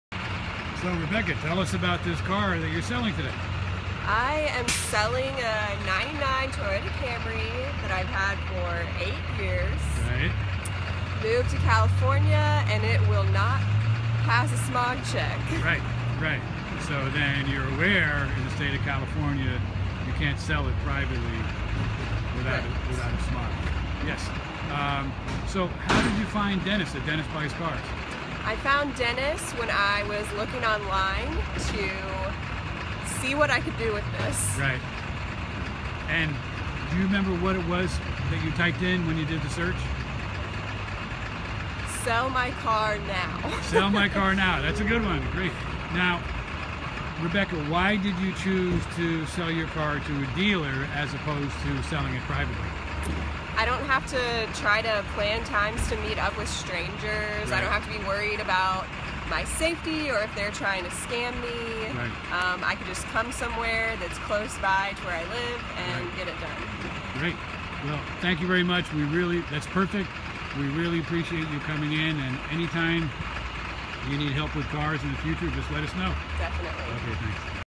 Sell My Car Now
Rebecca tell us about this car that you are selling today? I am selling a 1999 Toyota Camry that I've had for 8 years. I moved to California and if will not pass a smog check. You are aware that in the state of California you can't sell it privately without a smog? Correct. So how did you find Dennis at Dennisbuyscars? I found Dennis when I was looking online to see what I could do with this. Do you remember what it was that you typed in when you did the search. Sell my car now. Now Rebecca why did you sell your car to a dealer as oppose to selling it privately? I don't have to plan times to meet up with a stranger. I don't have to be worry about my safety or if they're trying to scam me. I could just come somewhere, where is close by from where I live and get it done. Thank you very much we really appreciate you coming in. Anytime that you need help with cars in the future just let us know.